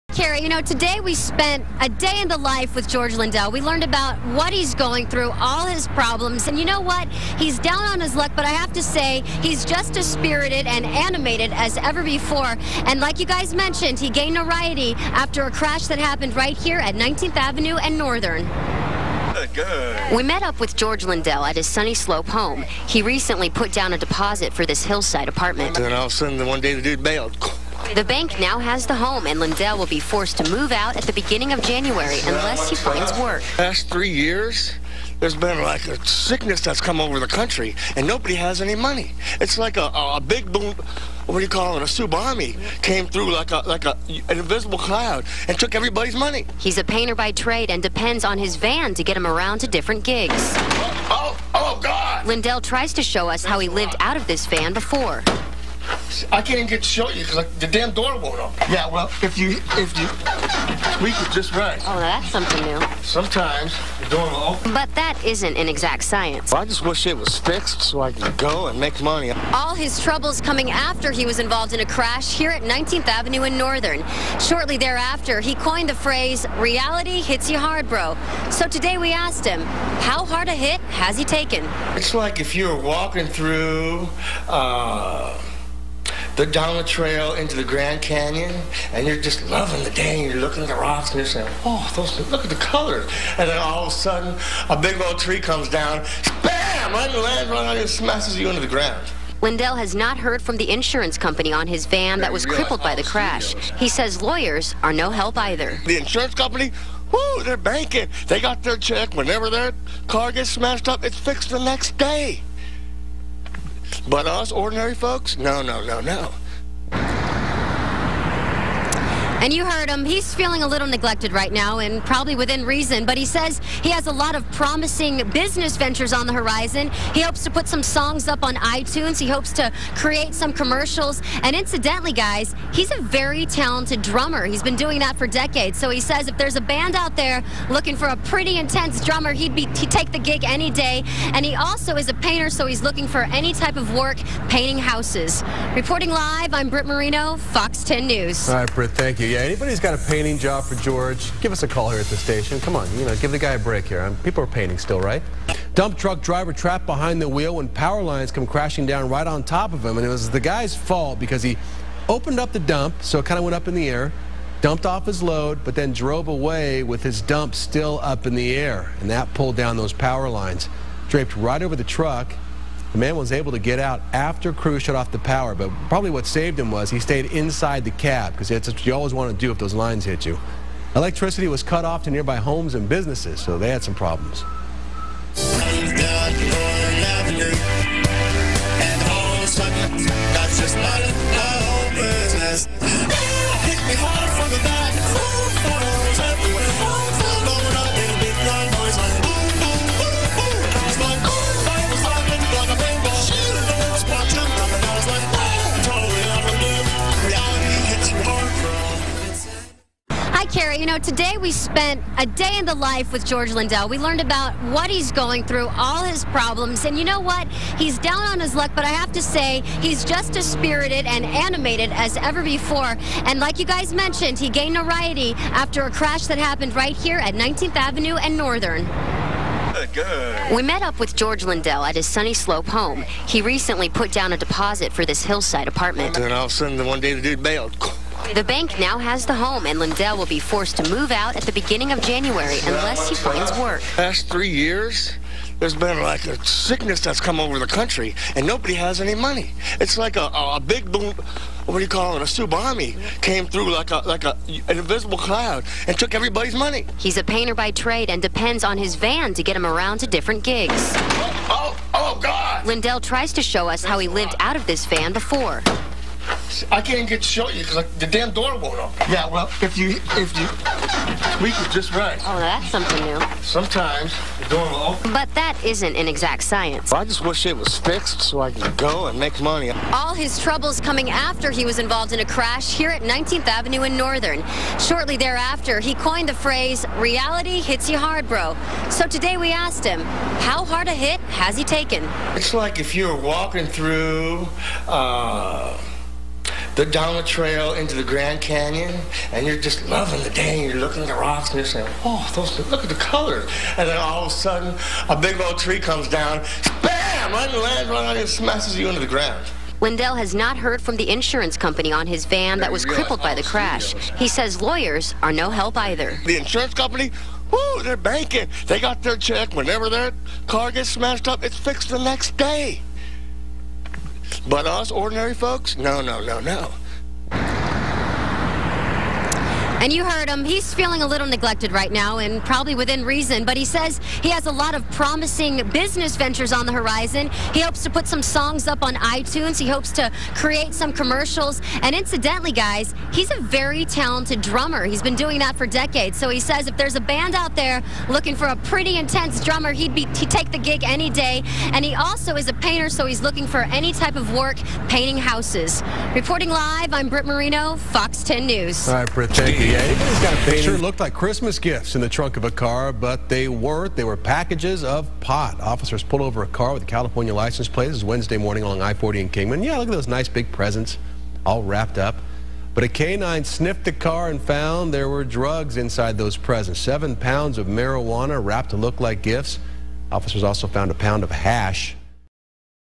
E-news - Reality Hits You Hard Bro (George Lindell) New Interview 12/30/ 2011
New Interview 12/30/ 2011. George is interviewed again! He tells the story of a huge "TsuBomby" coming through and taking everyone's money! His New Years plan is to release his new song and app, which will be posted when it is released!
 Video taken from My FoxPhoenix.com